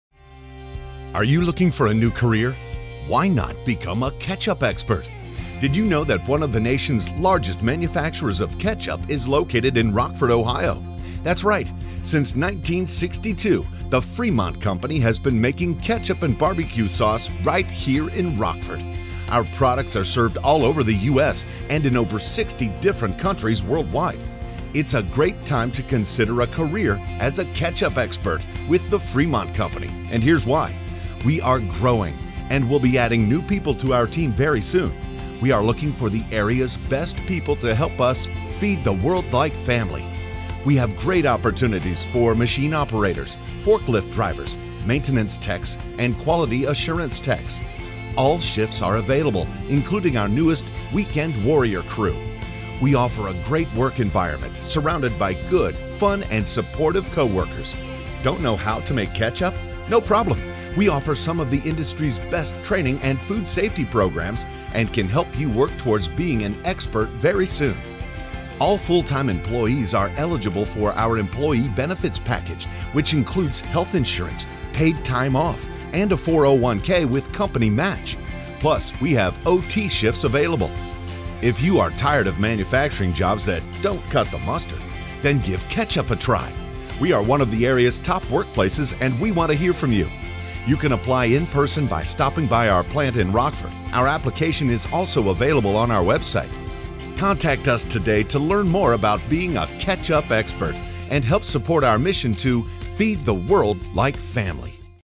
The Fremont Company - Rockford
Our Rockford, OH facility is hiring! Join our team and be a Ketchup Expert!